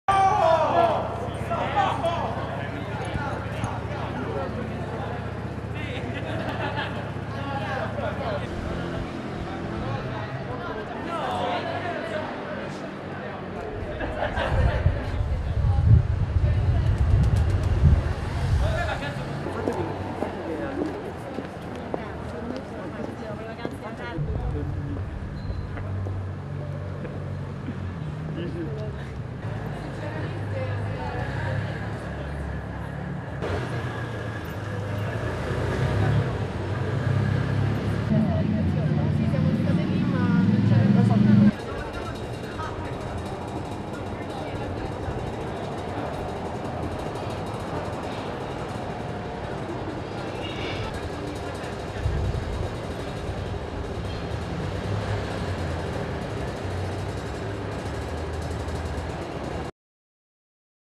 L’uscita da scuola a Roma nel primo giorno di ritorno in classe
(Agenzia Vista) - Roma, 18 Gennaio 2021 - Ecco le immagini dell’uscita da scuola al Liceo Ennio Quirino Visconti di Roma, il giorno del rientro alla didattica in presenza dopo la pausa iniziata a Novembre.